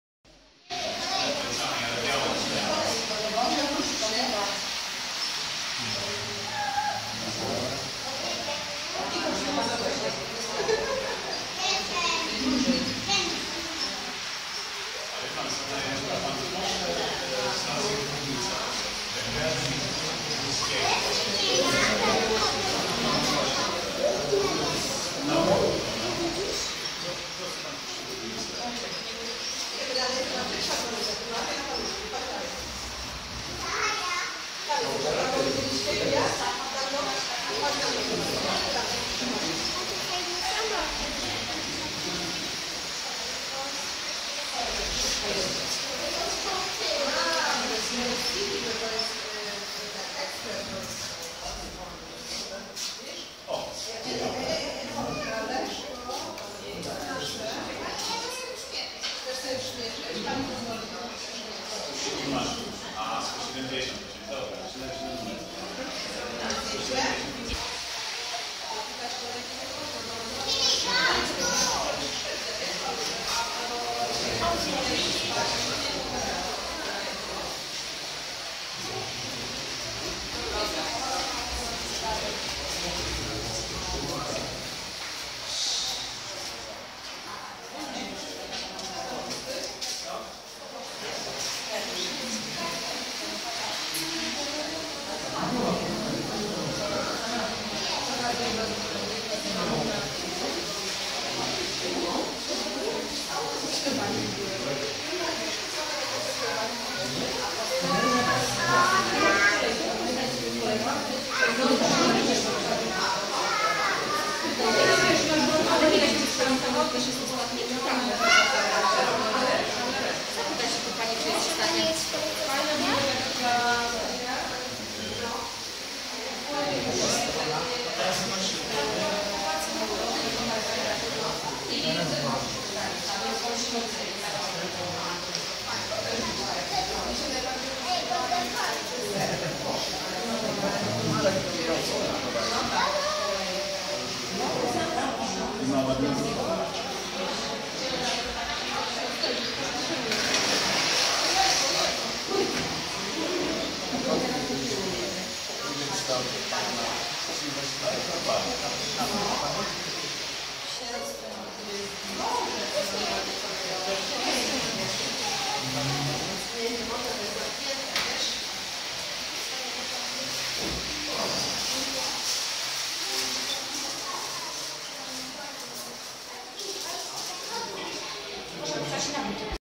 Spotkanie przy TMM - Grudziądz 2017. Mała makieta H0.
Spotkanie przy Toruńskiej Makiecie Modułowej w Grudziądzu w dniach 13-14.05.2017.